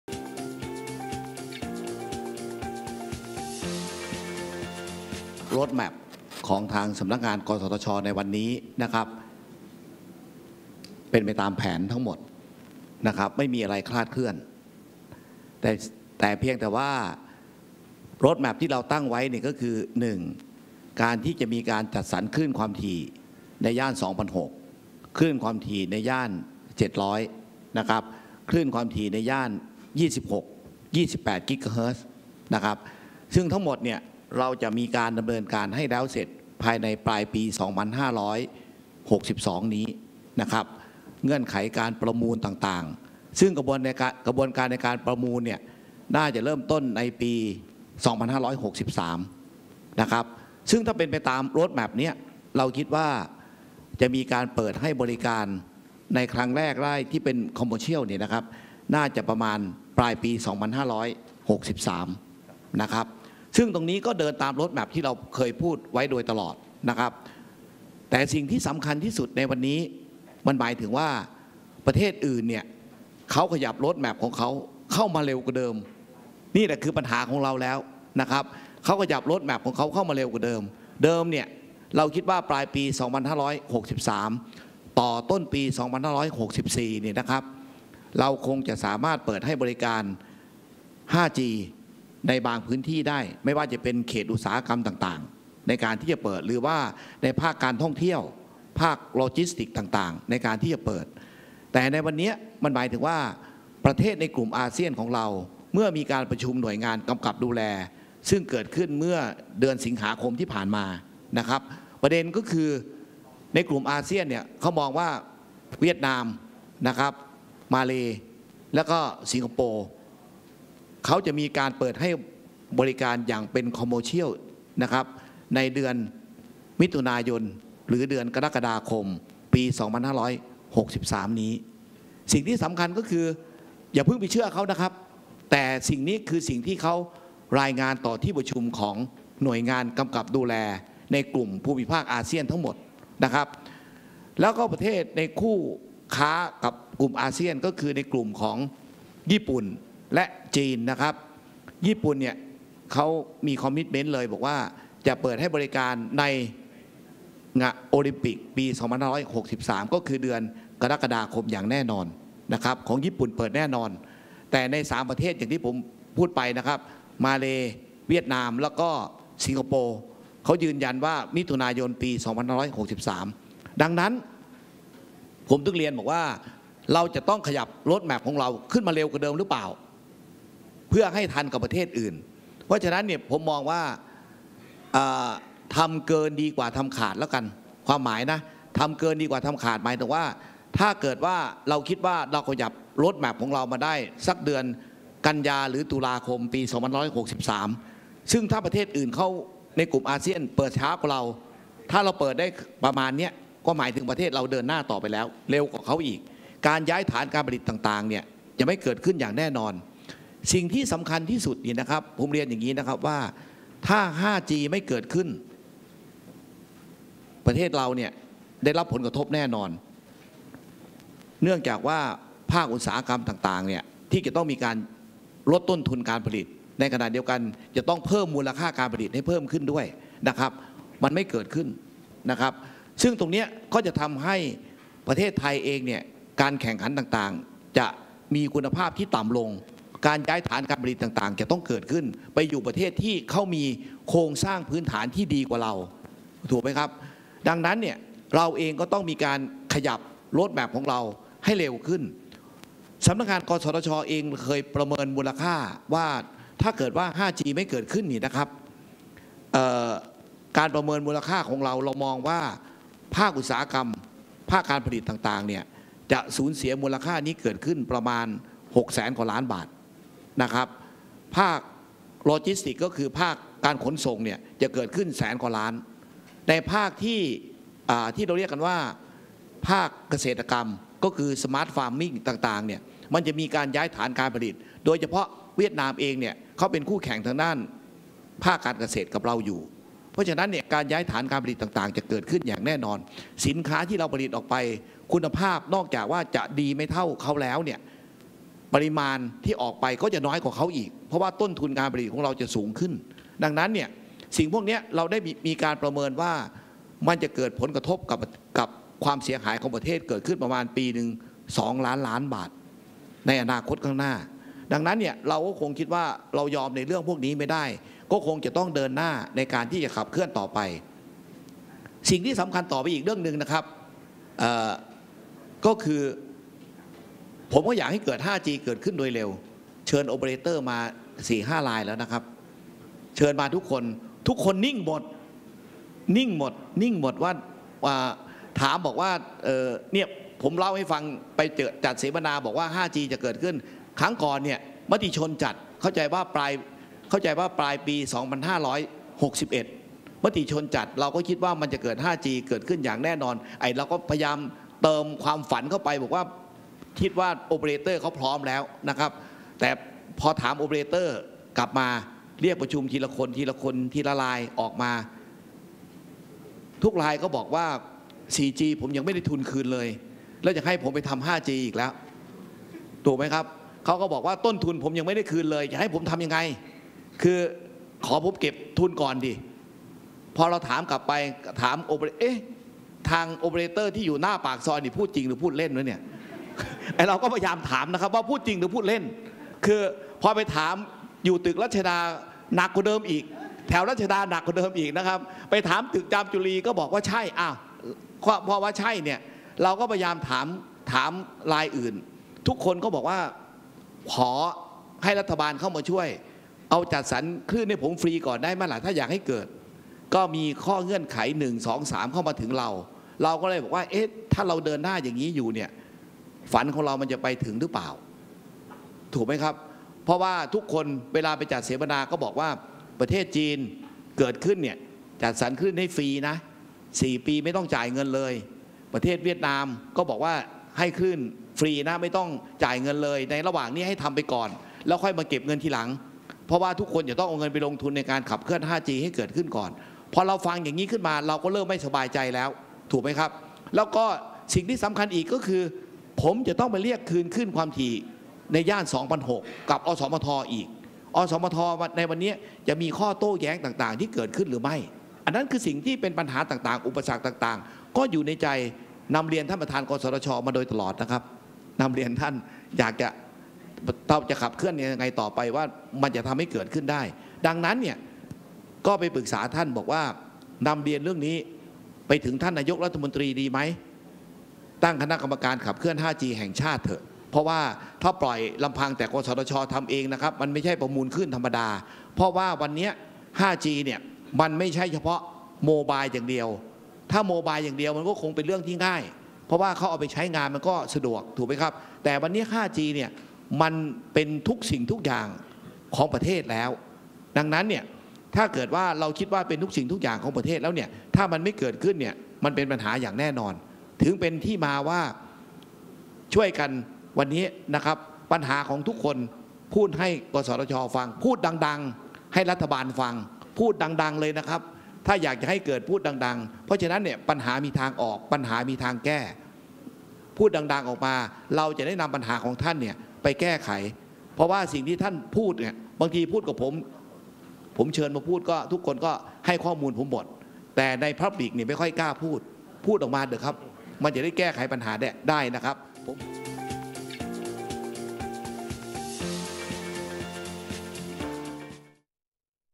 การจัดสรรคลื่นความถี่ ยังเป็นไปตามแผน
• นายฐากร ตัณฑสิทธิ์ เลขาธิการ กสทช.
เสวนา กระชากปม 5G ไปไม่ถึงฝัน